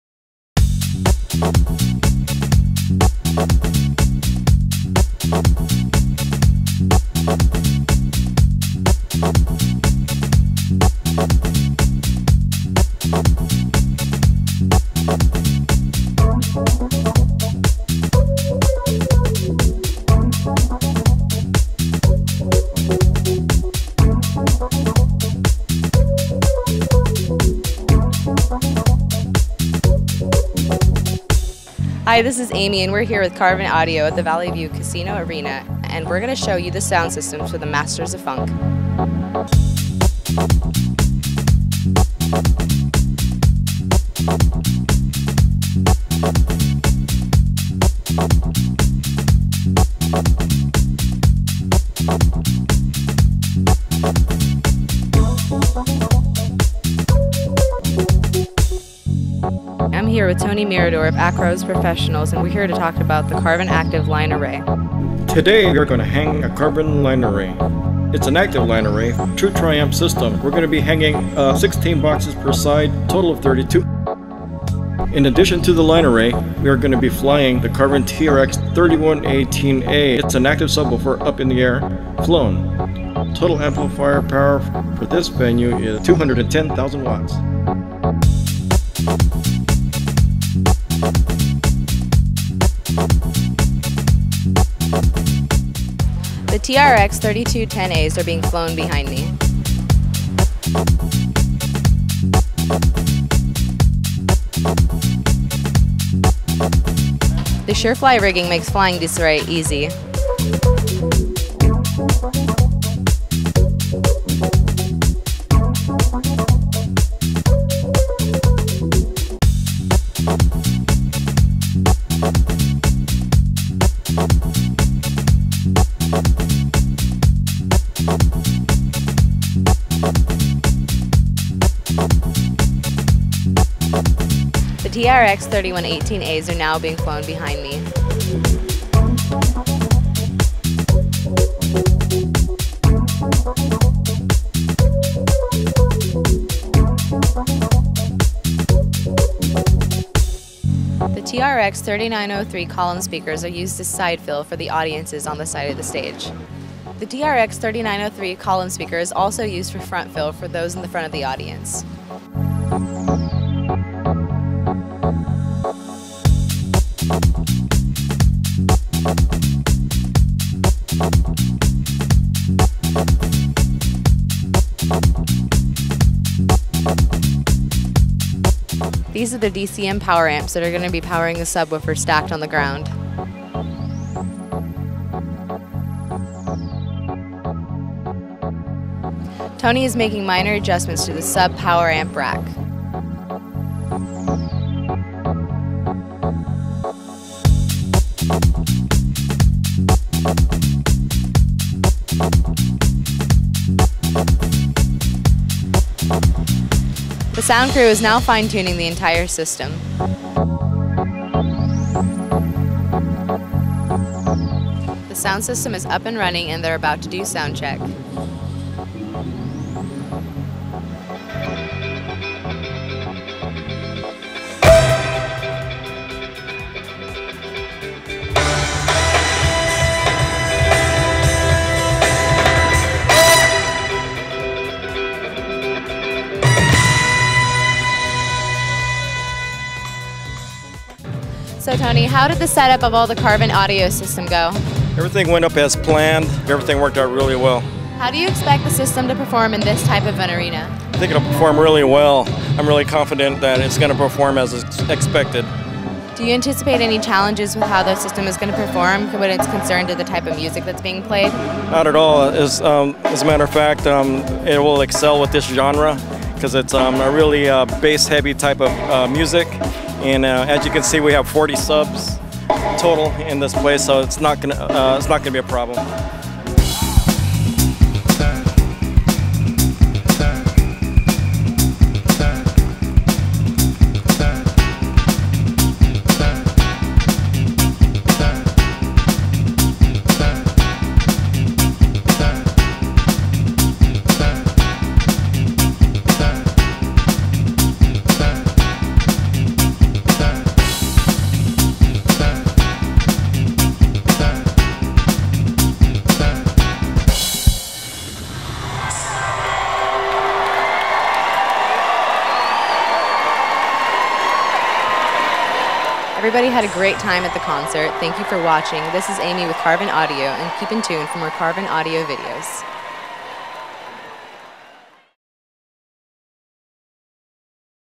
Carvin Audio Masters of Funk Show Report - San Diego Sports Arena - Valley View Casino Center
Akroz Professional was the production company for the Masters of Funk concert when it stopped in San Diego. The Carvin Audio TRx3210A Line Array was flown in the Arena formerly known as the San Diego Sports Arena - now called The Valley View Casino Center. The TRx3118A subs were flown along side the Line Array. See the full set up of the system from load-in to show.